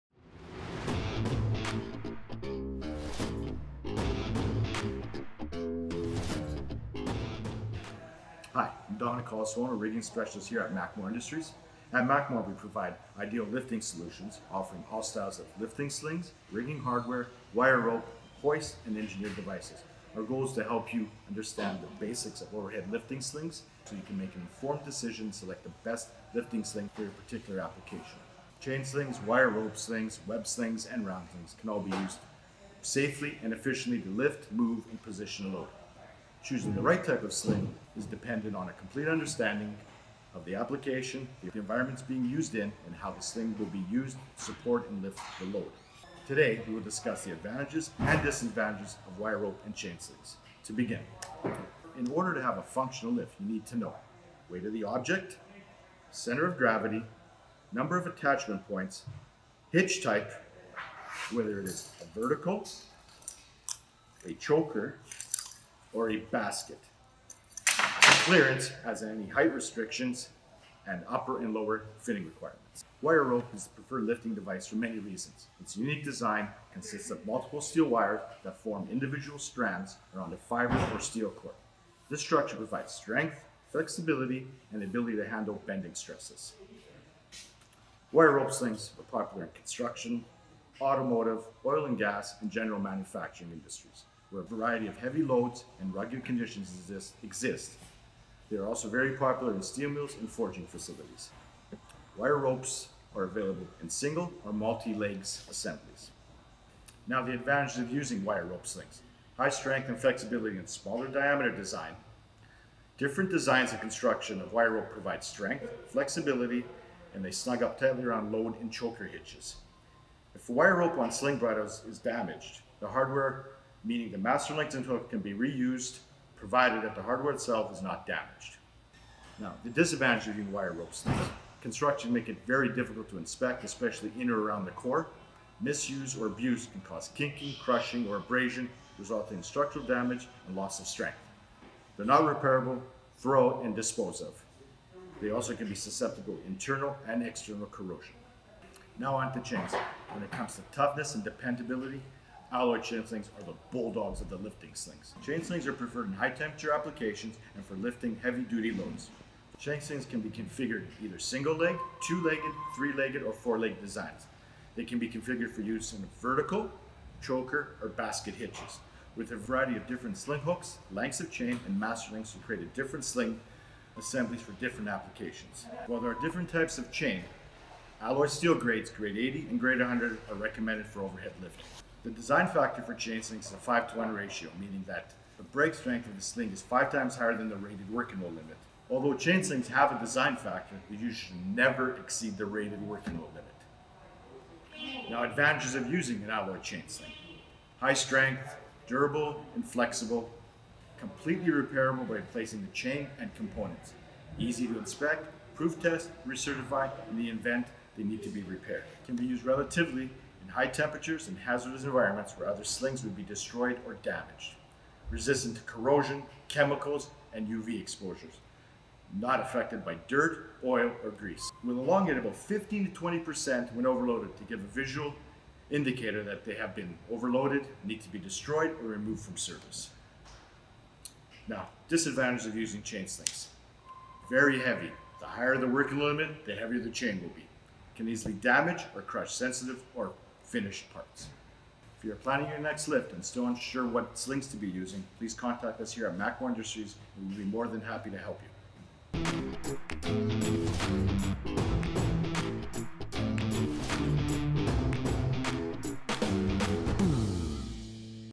What is the Difference Between Wire Rope and Chain Slings? - Rigging 101
MacMor's Rigging Specialist Dom Colasuonno discusses the difference between wire rope and chain assemblies. Dom also gives free advice on planning your next rigging assembly.